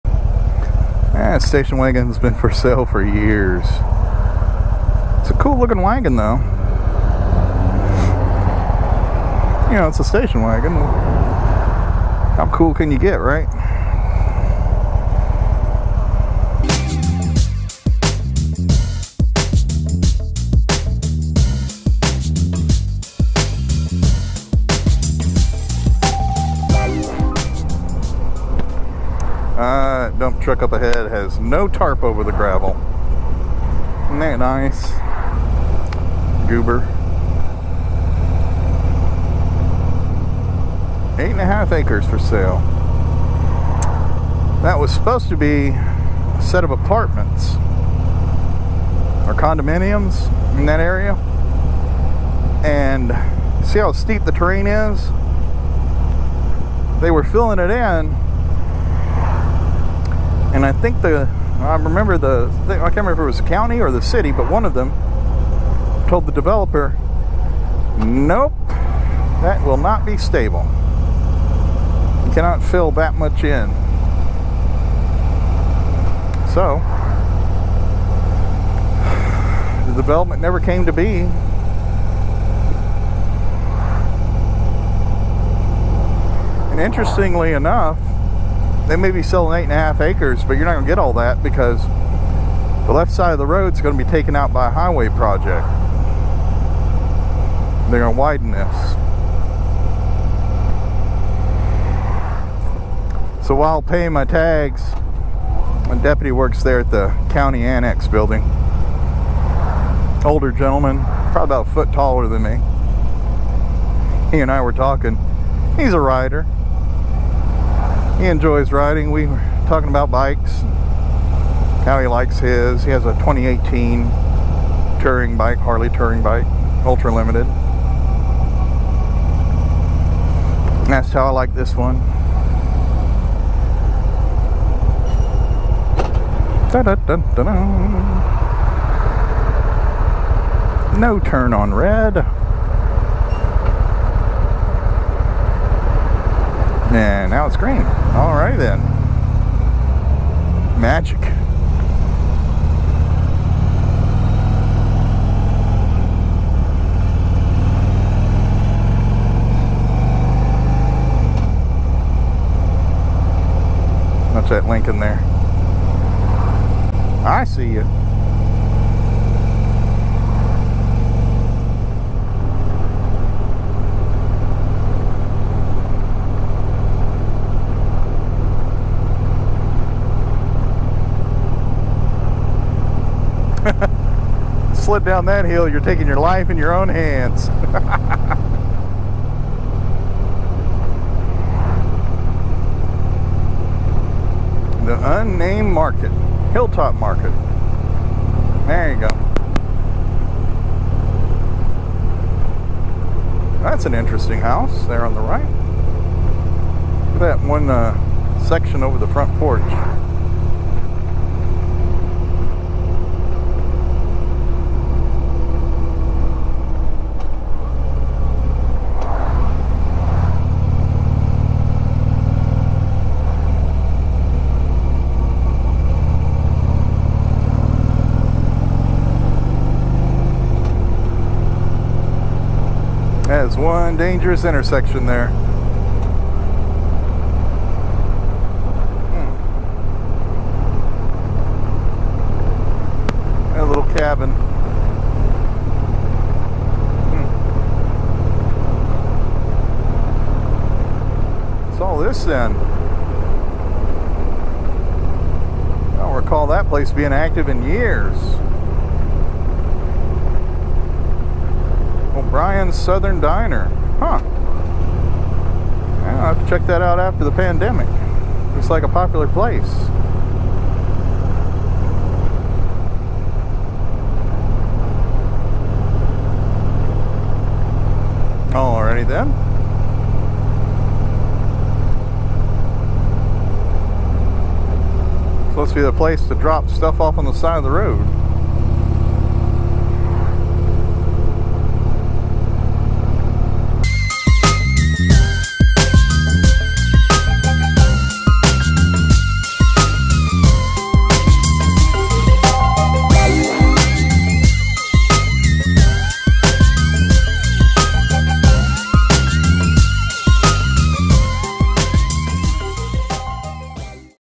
Random thoughts from the saddle
Riding clears the mind and you get to see some cool things along the way.

Join me a nice ride with some random observations.